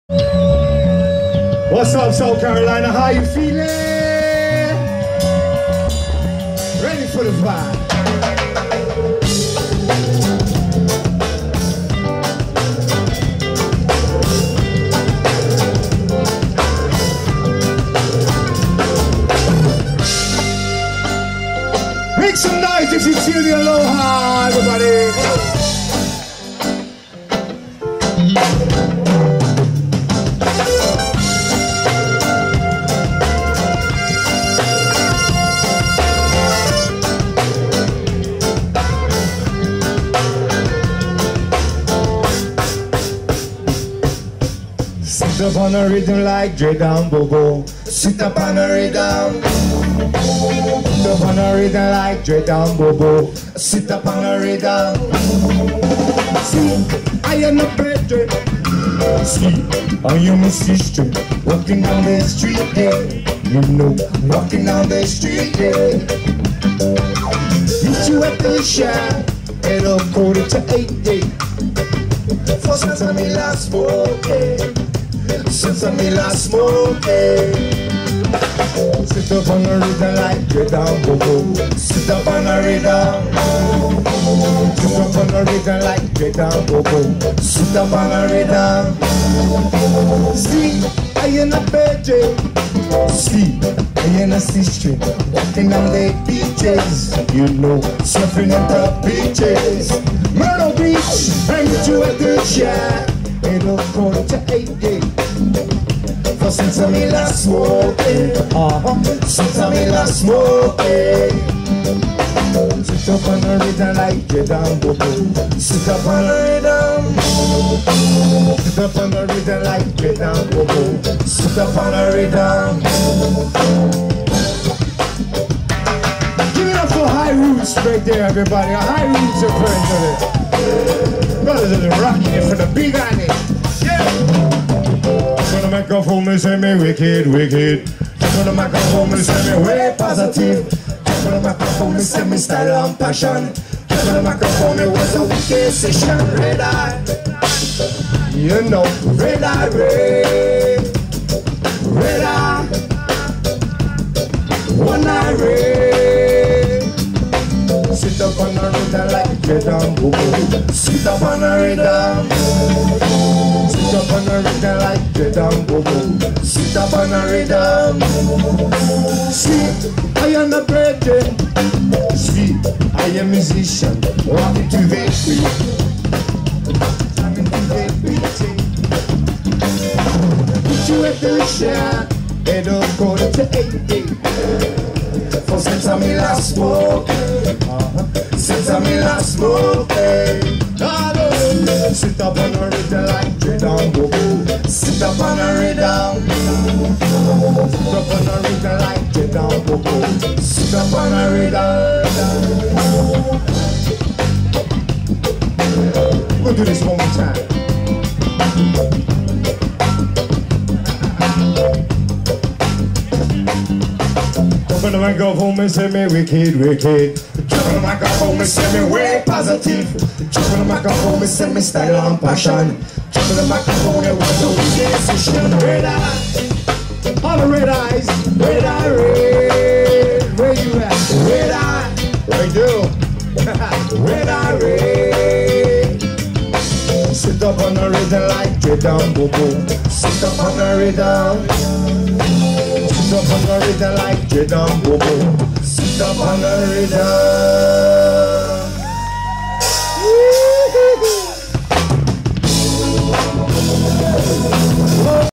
Natty Vibes (Live) @ Boathouse Myrtle Beach
Natty Vibes performing at Boathouse Waterway Bar & Grill Myrtle Beach.

Do you know the TITLE OF THE TRACK?